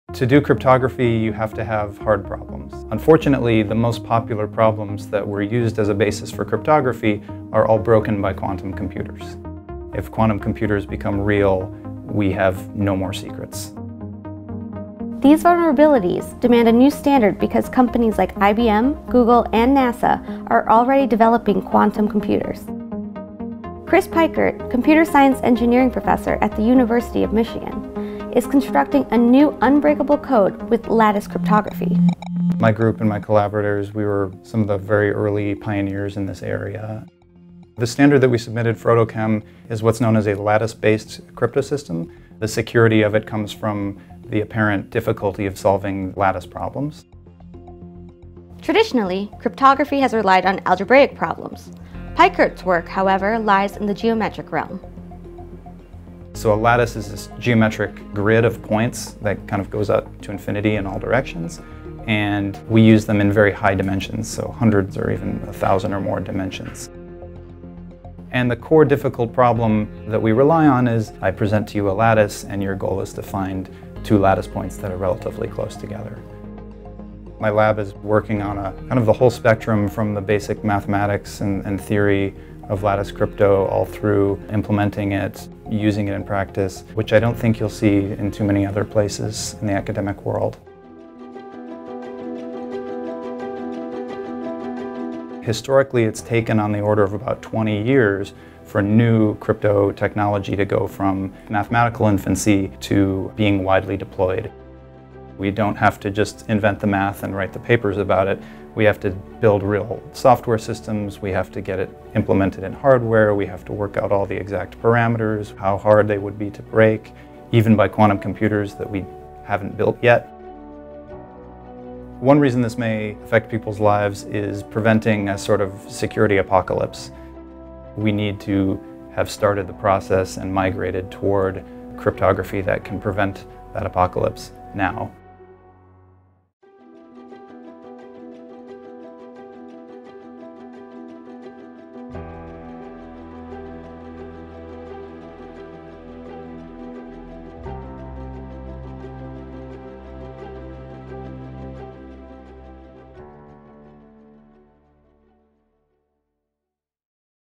Lattice cryptography: A new unbreakable code
Computer science researchers are creating a new standard with lattice cryptography for a post-Moore's law world, where quantum computing is standard and traditional algebraic security methods would be easily broken.

While traditional cryptography methods have relied on algebra, lattice cryptography utilizes the geometric realm–going in different dimensions in many different directions. The result is a much more difficult to crack system that provides greater security for everyone.

Christopher Peikert, the Patrick C Fischer Development Professor of Theoretical Computer Science and associate professor of electrical engineering and computer science, has research interests including cryptography, lattices, coding theory, algorithms, and computational complexity.

*This video is an update to a previous version that incorrectly referred to conventional cryptography as relying on the factoring of prime numbers, instead of factoring of numbers that are the product of two large primes.